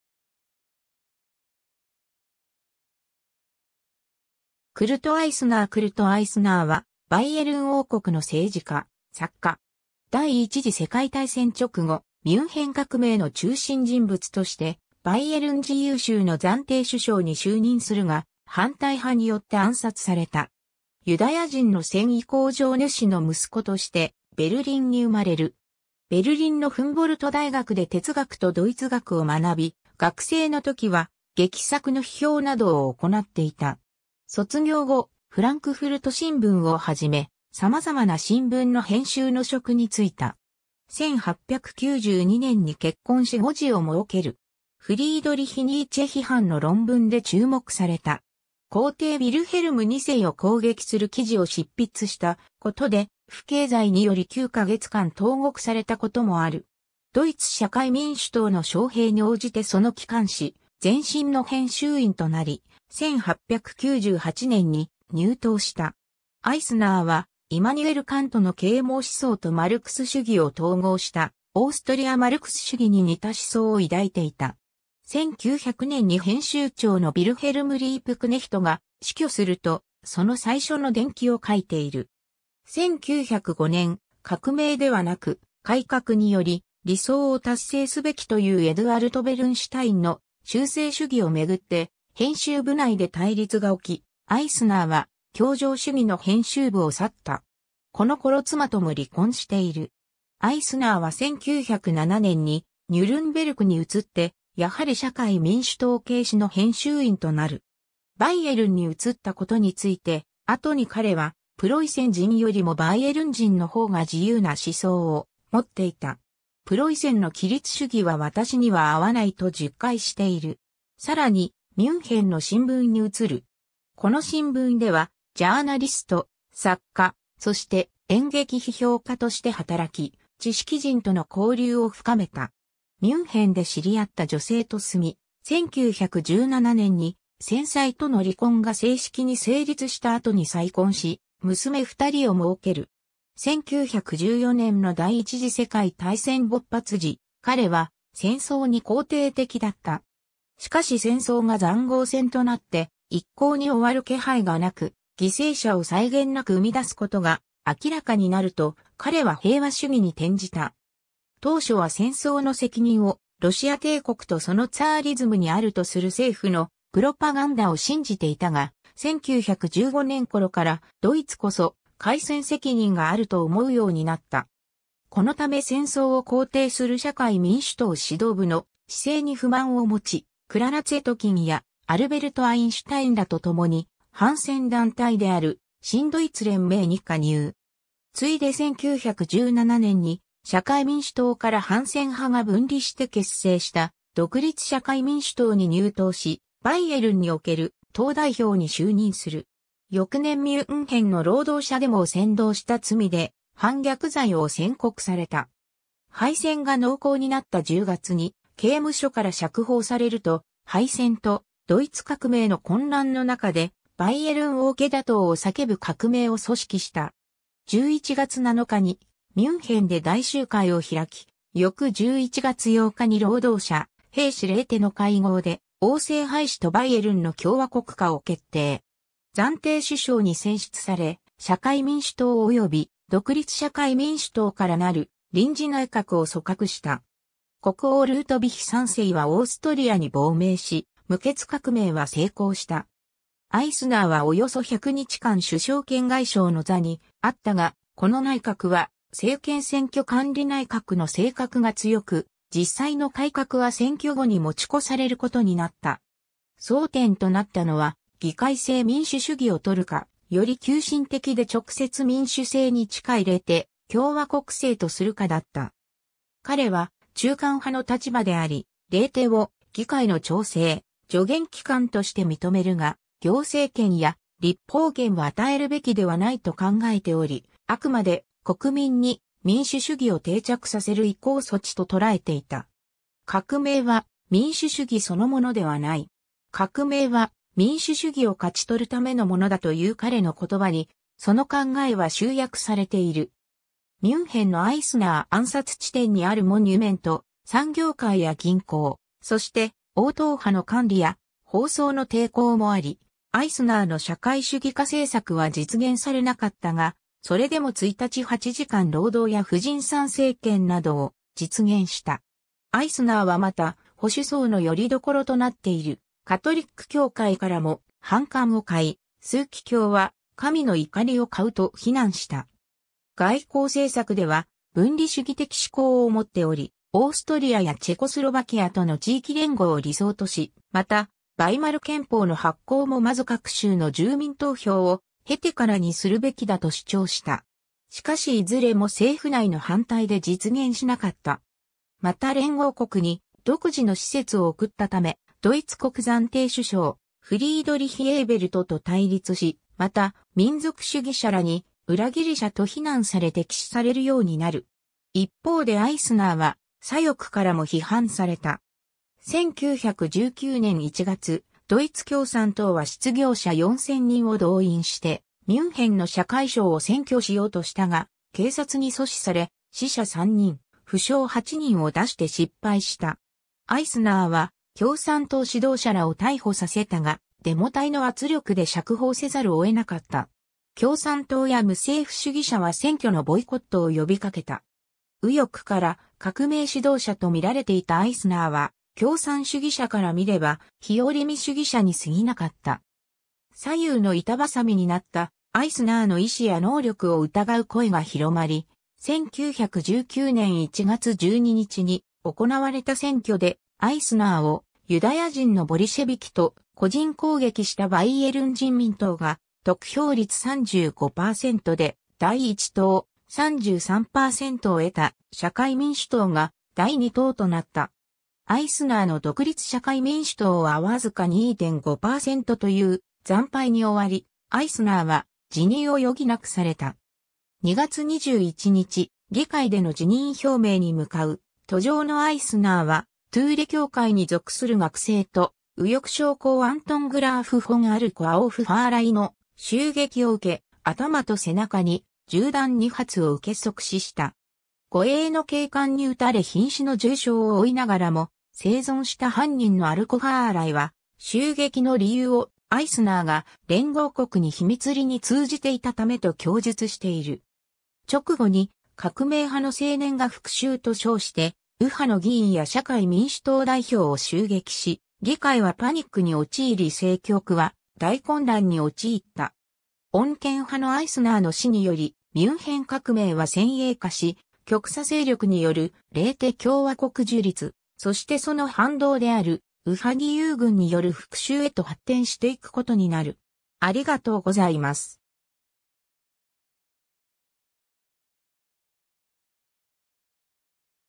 クルト・アイスナー
クルト・アイスナー（1919年） クルト・アイスナー（Kurt Eisner, 1867年5月14日 - 1919年2月21日）は、バイエルン王国の政治家、作家。
第一次世界大戦直後、ミュンヘン革命の中心人物としてバイエルン自由州の暫定首相に就任するが、反対派によって暗殺された。
 ユダヤ人の繊維工場主の息子としてベルリンに生まれる。
ベルリンのフンボルト大学で哲学とドイツ学を学び、学生の時は劇作の批評などを行っていた。
卒業後『フランクフルト新聞』をはじめ、さまざまな新聞の編集の職に就いた。
1892年に結婚し5児をもうける。
フリードリヒ・ニーチェ批判の論文で注目された。
皇帝ヴィルヘルム2世を攻撃する記事を執筆したことで、不敬罪により9か月間投獄されたこともある。
ドイツ社会民主党の招聘に応じてその機関紙『前進』(Vorwärts) の編集員となり、1898年に入党した。
アイスナーはイマニュエル・カントの啓蒙思想とマルクス主義を統合したオーストリア・マルクス主義に似た思想を抱いていた。
1900年に編集長のヴィルヘルム・リープクネヒトが死去すると、その最初の伝記を書いている。
 1905年、革命ではなく改革により理想を達成すべきというエドゥアルト・ベルンシュタインの修正主義をめぐって編集部内で対立が起き、アイスナーは教条主義の編集部を去った。
この頃妻とも離婚している。
アイスナーは1907年にニュルンベルクに移ってやはり社会民主党系紙の編集員となる。
バイエルンに移ったことについて、後に彼は「プロイセン人よりもバイエルン人のほうが自由な思想を持っていた。プロイセンの規律主義は私には合わない」と述懐している。
さらにミュンヘンの新聞に移る。
この新聞ではジャーナリスト、作家、そして演劇批評家として働き、知識人との交流を深めた。
ミュンヘンで知り合った女性と住み、1917年に先妻との離婚が正式に成立した後に再婚し娘2人をもうける。
 1914年の第一次世界大戦勃発時、彼は戦争に肯定的だった。
しかし戦争が塹壕戦となって一向に終わる気配がなく、犠牲者を際限なく生み出すことが明らかになると、彼は平和主義に転じた。
当初は戦争の責任をロシア帝国とそのツァーリズムにあるとする政府のプロパガンダを信じていたが、1915年頃からドイツこそ開戦責任があると思うようになった。
このため戦争を肯定する社会民主党指導部の姿勢に不満を持ち、クララ・ツェトキンやアルベルト・アインシュタインらと共に反戦団体である「新ドイツ連盟」に加入。
ついで1917年に社会民主党から反戦派が分離して結成した独立社会民主党に入党し、バイエルンにおける党代表に就任する。
翌年ミュンヘンの労働者デモを煽動した罪で反逆罪を宣告された。
 敗戦が濃厚になった10月に刑務所から釈放されると、敗戦とドイツ革命の混乱の中でバイエルン王家打倒を叫ぶ革命を組織した。
11月7日にミュンヘンで大集会を開き、翌11月8日に労働者・兵士レーテ（評議会）の会合で王制廃止とバイエルンの共和国化を決定。
暫定首相に選出され、社会民主党および独立社会民主党からなる臨時内閣を組閣した。
国王ルートヴィヒ3世はオーストリアに亡命し、無血革命は成功した。
アイスナーはおよそ100日間首相兼外相の座にあったが、この内閣は制憲選挙管理内閣の性格が強く、実際の改革は選挙後に持ち越されることになった。
争点となったのは議会制民主主義を採るか、より急進的で直接民主制に近いレーテ共和国制とするかだった。
彼は中間派の立場であり、レーテを議会の調整・助言機関として認めるが、行政権や立法権は与えるべきではないと考えており、あくまで「国民に民主主義を定着させる移行措置」と捉えていた。
「革命は民主主義そのものではない。革命は民主主義を勝ち取るためのものだ」という彼の言葉に、その考えは集約されている。
 ミュンヘンのアイスナー暗殺地点にあるモニュメント 産業界や銀行、そして王党派の官吏や法曹の抵抗もあり、アイスナーの社会主義化政策は実現されなかったが、それでも一日8時間労働や婦人参政権などを実現した。
アイスナーはまた、保守層の拠り所となっているカトリック教会からも反感を買い、枢機卿は「神の怒りを買う」と非難した。
外交政策では分離主義的志向をもっており、オーストリ...